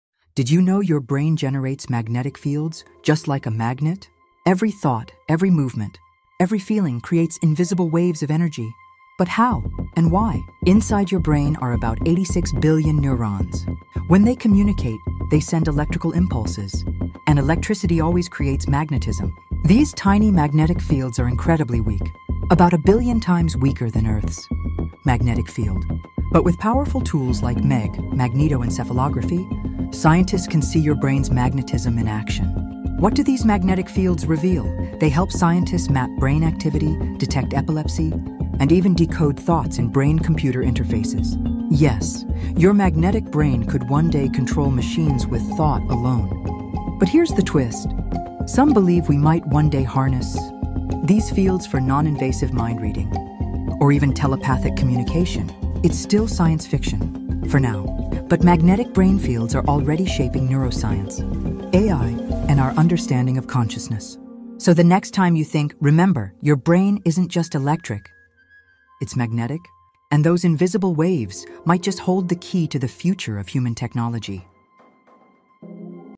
Your Brain Creates Magnetic Fields – And It’s Wilder Than You Think!"
🧲 Did you know your brain creates magnetic fields every time you think?
Yes—it’s not just electrical. Your neurons fire tiny impulses that generate weak, yet detectable magnetic fields. Using powerful tools like MEG (Magnetoencephalography), scientists can now map your thoughts, predict seizures, and even decode intentions for brain-computer interfaces.

In this video, we explore how your brain’s magnetic field works, why it matters, and what it could mean for the future of neuroscience and mind-controlled technology. 🧬🧠

🔍 Topics covered:

How neurons generate magnetic fields

What MEG scanners actually detect

Brain-computer interface breakthroughs

Future tech: telepathy and magnetic mind reading?

👇 Leave a comment: What do YOU think we’ll be able to do with brain magnetism in 10 years?